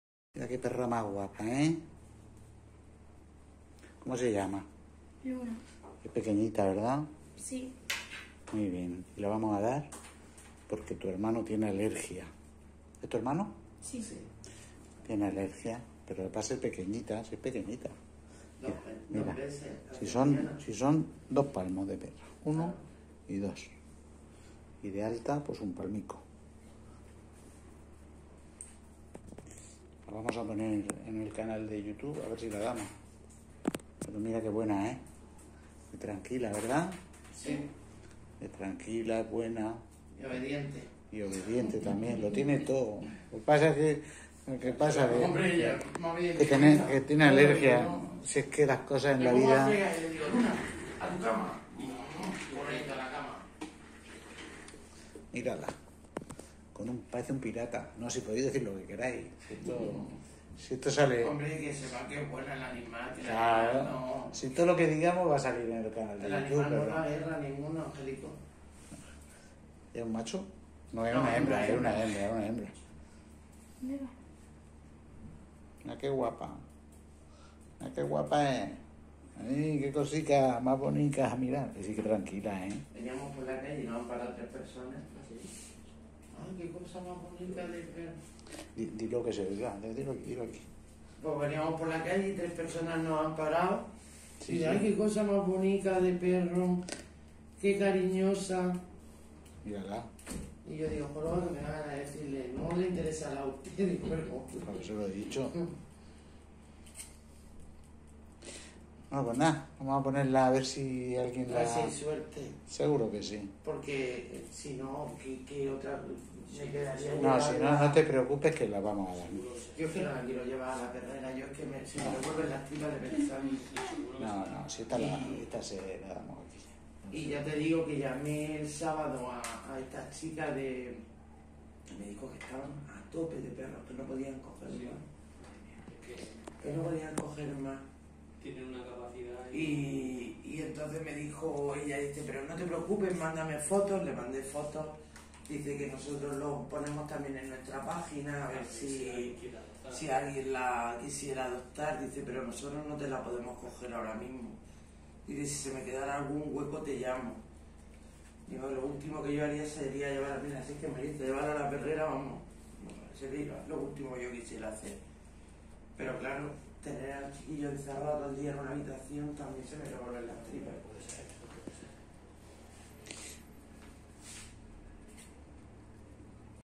un Cachorro, raza pequeña, Murcia, hoy 12 septiembre, 618396258
En adopción en clinica veterinaria Santa María de Gracia en Murcia capital.
Ven a la clinica y adopta los.